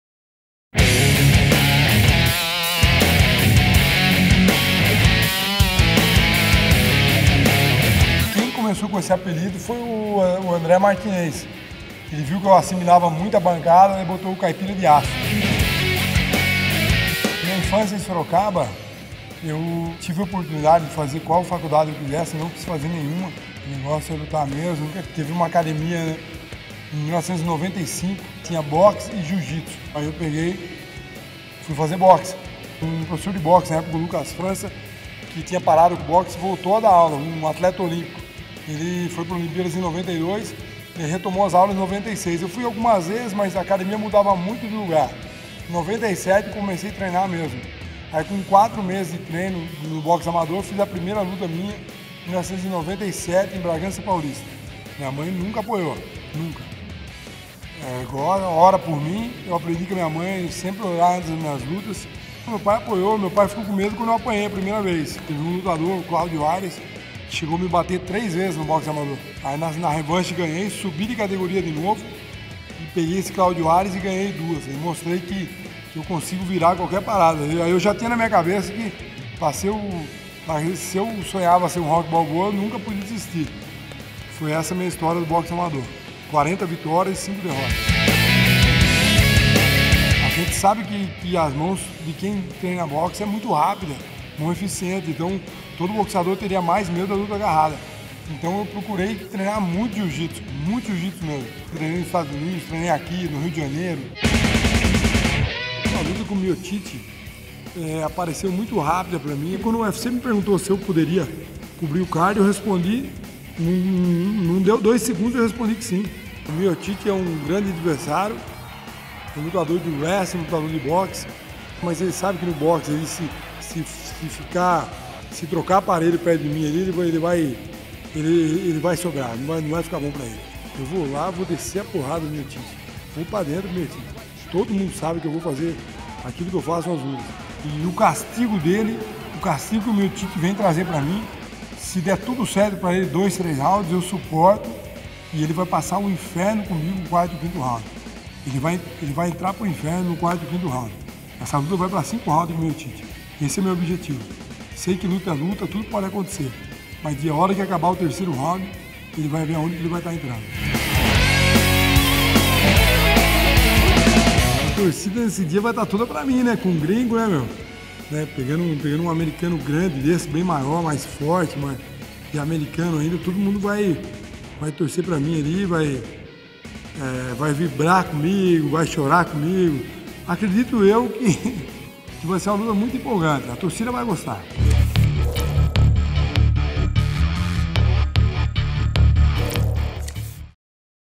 Fight Night Sao Paulo: Fabio Maldonado Pre-fight Interview
Fabio Maldonado, winner of three straight, previews his bout with Stipe Miocic at Fight Night Sao Paulo. Despite taking the fight on short notice, Maldonado is convince he will be able to beat Miocic up when they step into the Octagon on Saturday.